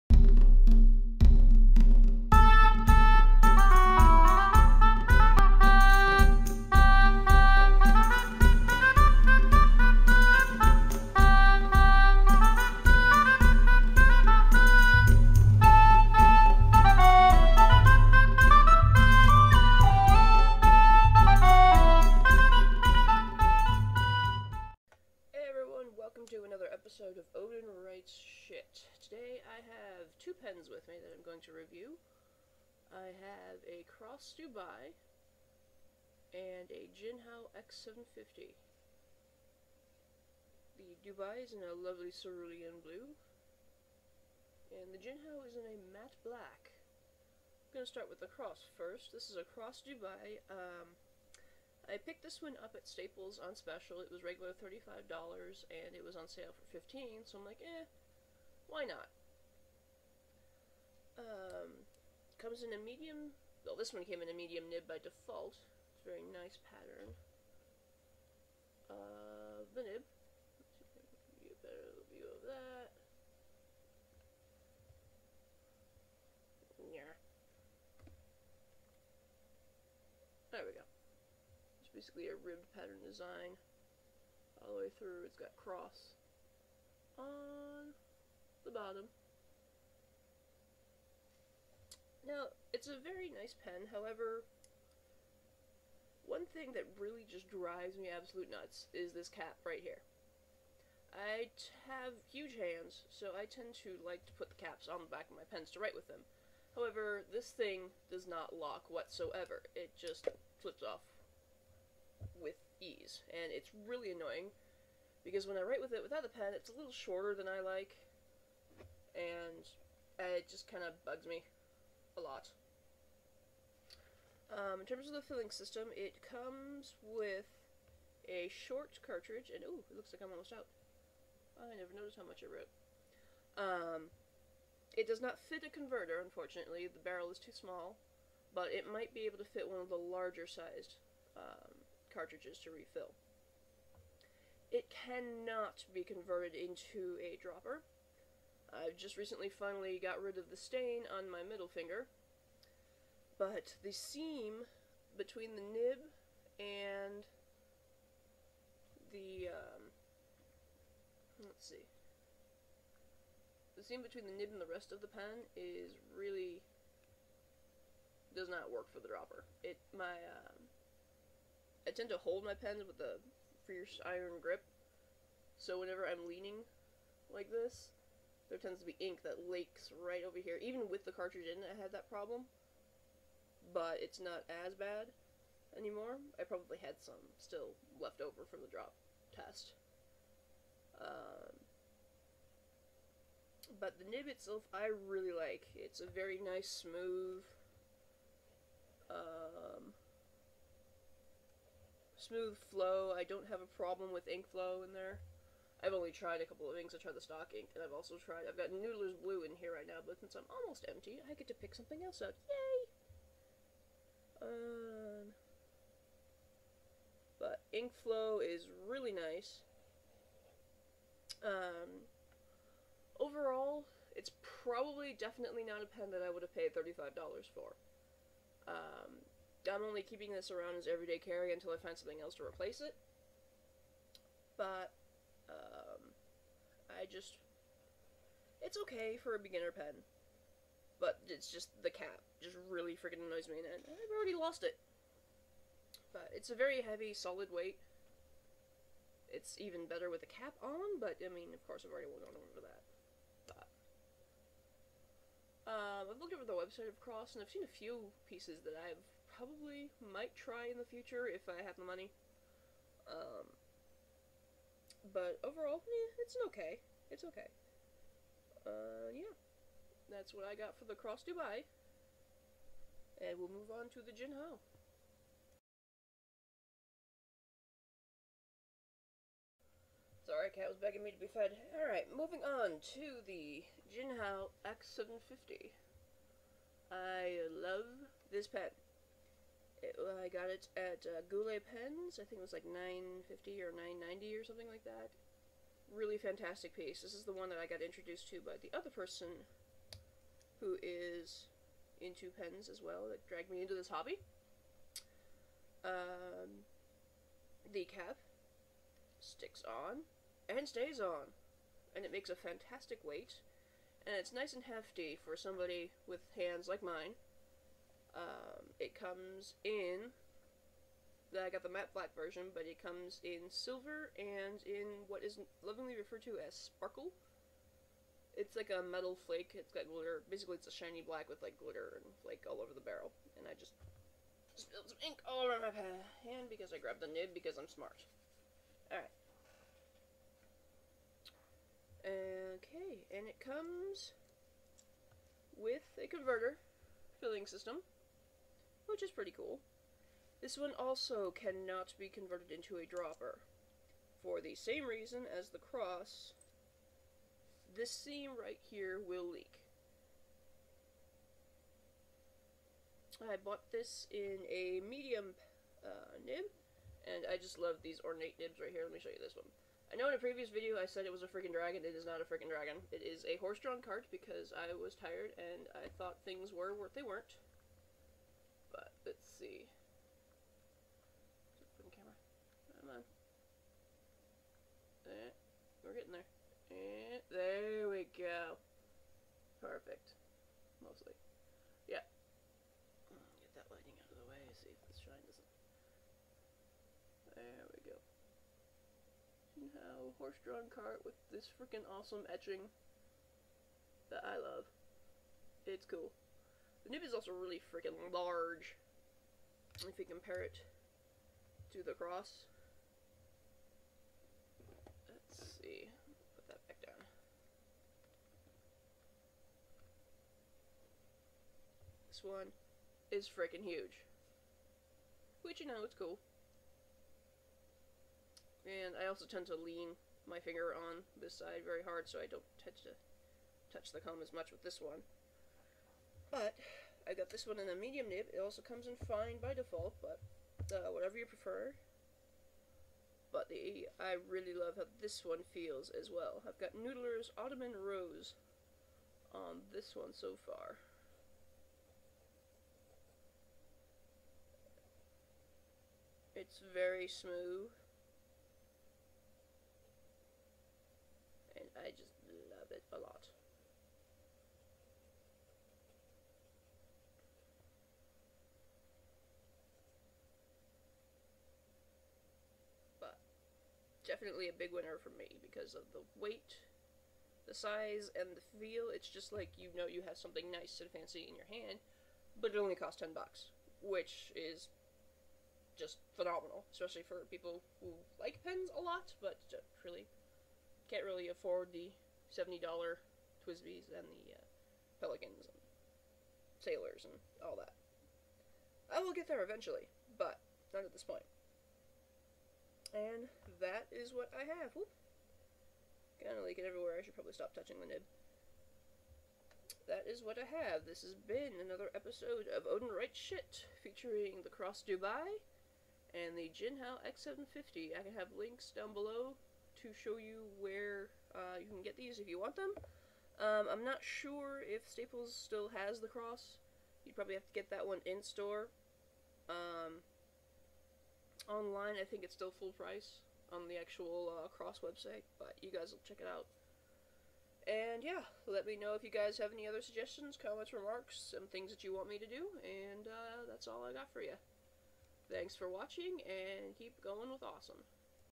Odin Writes S%^#: Cross Dubai vs. Jinhao x750
Today I am sharing with you a review on the Cross Dubai and the Jinhao x750. SOme basic started pens.

The Cross Dubai is still available on Staples for 15$. According to the site, the sale expires 12/31/2099 :/

Some Mild Language here so if you're easily offended.